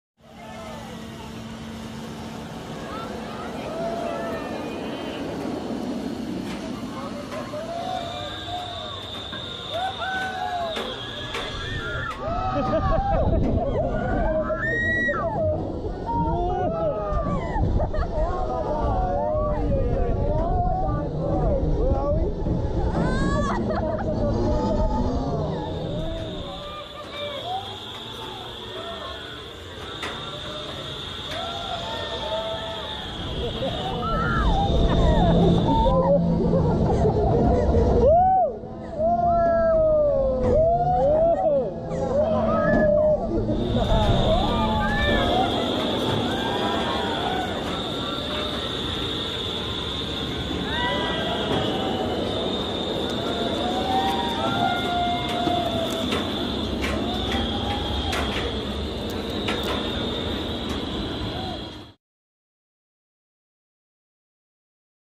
CYBORG Cyber Spin at Six Flags Great Adventure
Six Flags Great Adventure newest attraction, touted as its "most futuristic, jaw-dropping thrill ride," is here for you guys.